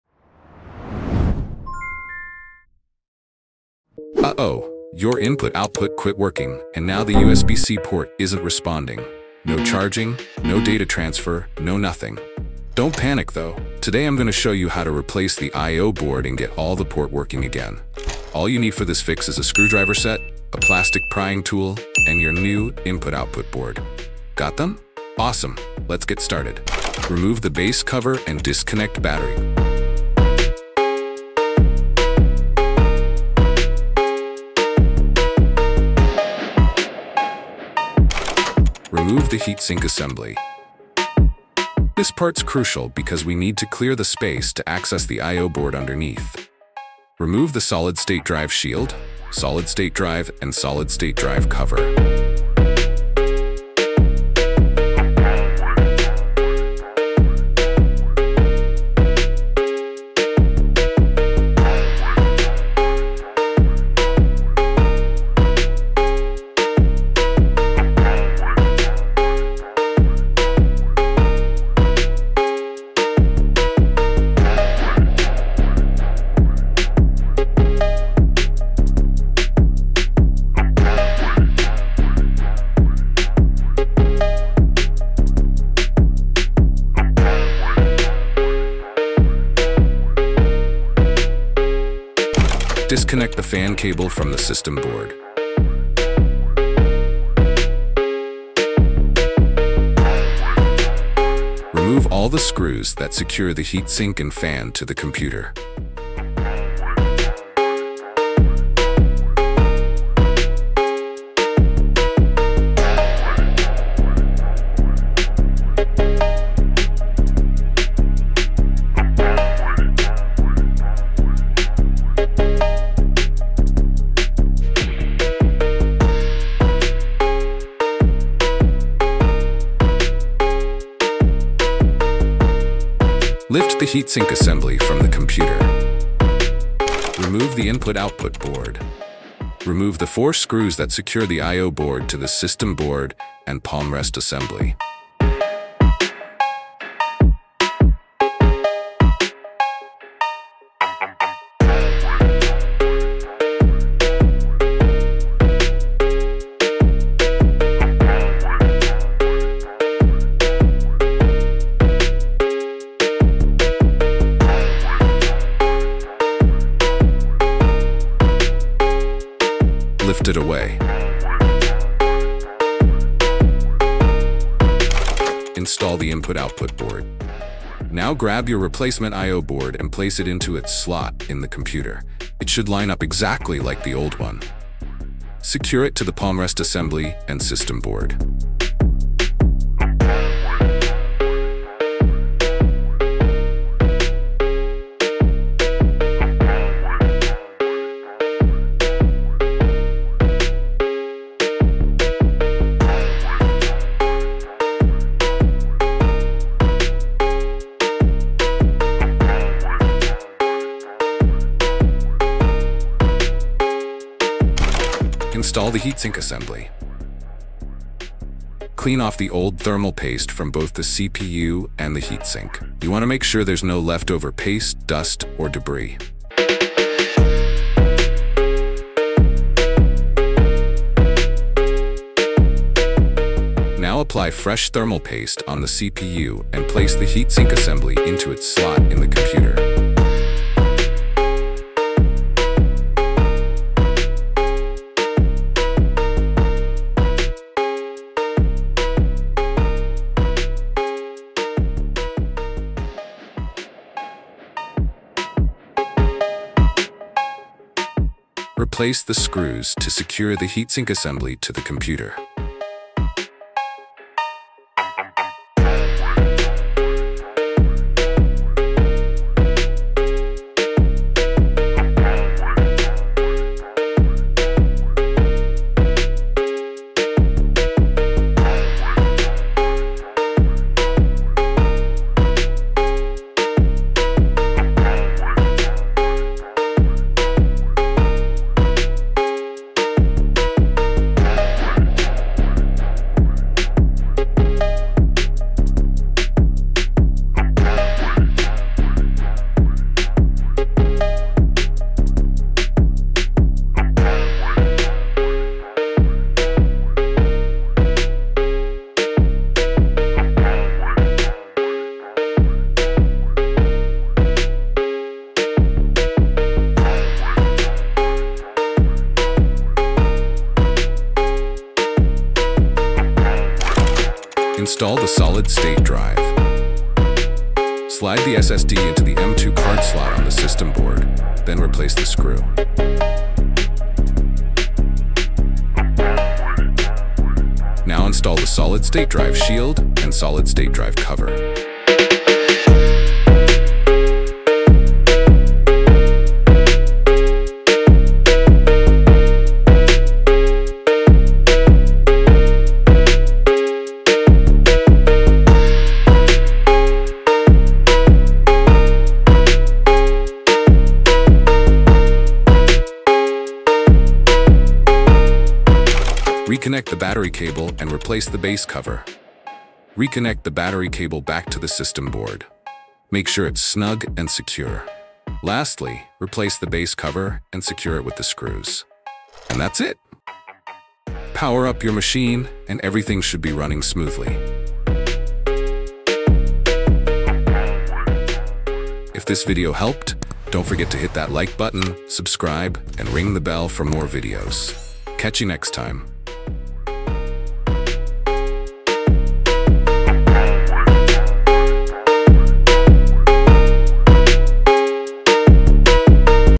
Dell Latitude 7420 I/O Board Failure? Here’s the Ultimate Fix!
Is your Dell Latitude 7420 acting up? Don’t worry! In this video, I’ll show you how to replace the I/O Board of your laptop quickly and easily. 🙌

🔧 What You’ll Learn:

- Simple tools you’ll need for the job 🔨
- How to safely disassemble your laptop 🛠️
- Step-by-step instructions to replace the I/O board 📚